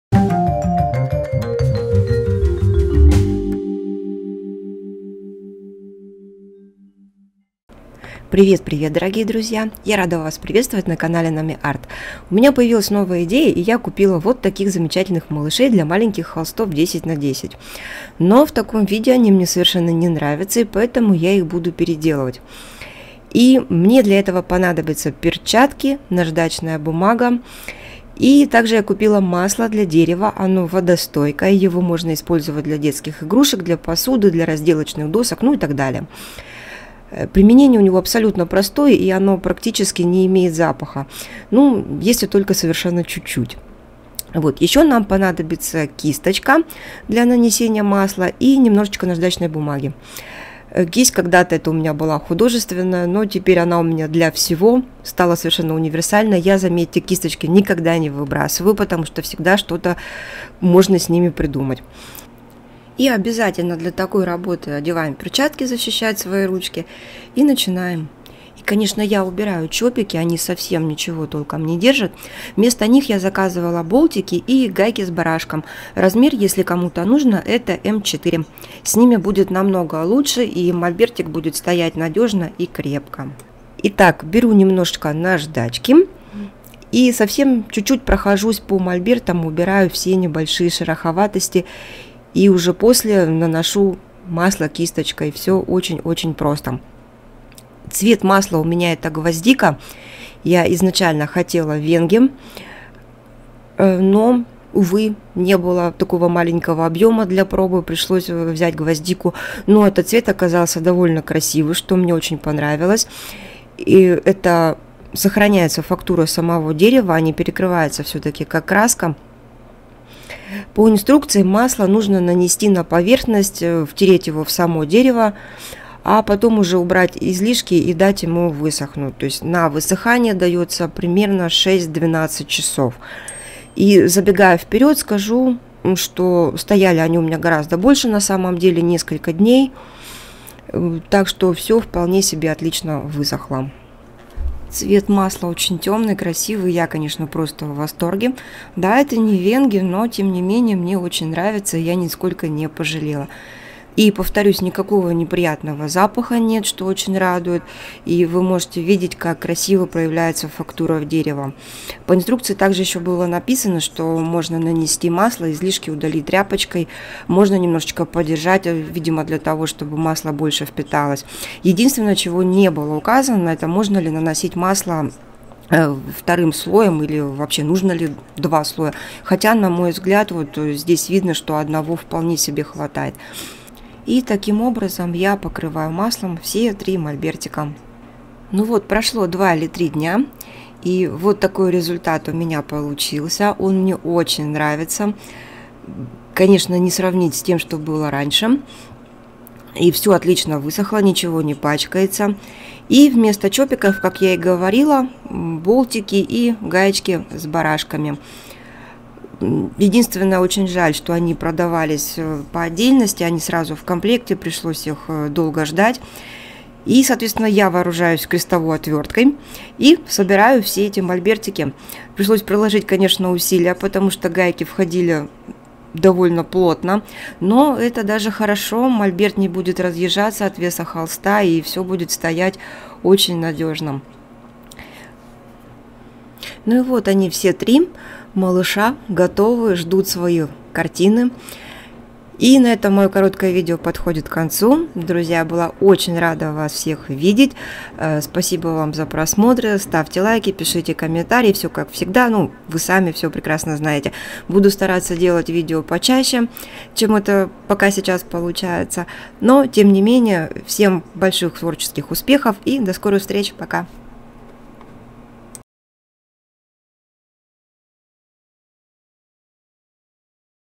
Творческая трансформация миниатюрных мольбертов.
Подготовьтесь к увлекательному путешествию в мир творчества! В этом видео я покажу, как я улучшила небольшие мольберты для миниатюр, придав им прочность и эстетическую привлекательность. Вы увидите, как я использовала маслом для дерева и еще кое что, делая их более надежными и красивыми. Присоединяйтесь ко мне в этом увлекательном процессе творческой трансформации!